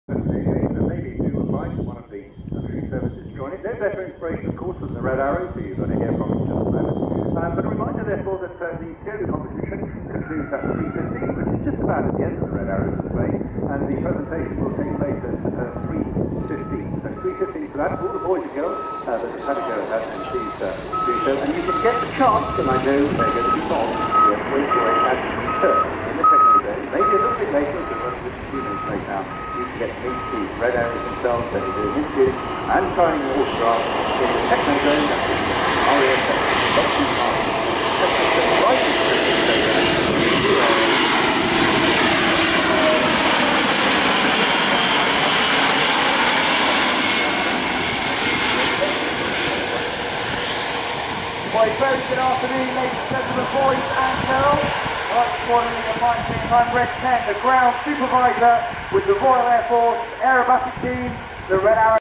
The Airbus A400M Atlas landing Royal Air Force Fairford Airport during RIAT
The Airbus A400M Atlas,[3][4] is a multi-national four-engine turboprop military transport aircraft.
RAF Fairford (IATA: FFD, ICAO: EGVA) is a Royal Air Force (RAF) station in Gloucestershire, England
Royal International Air Tattoo at RAF Fairford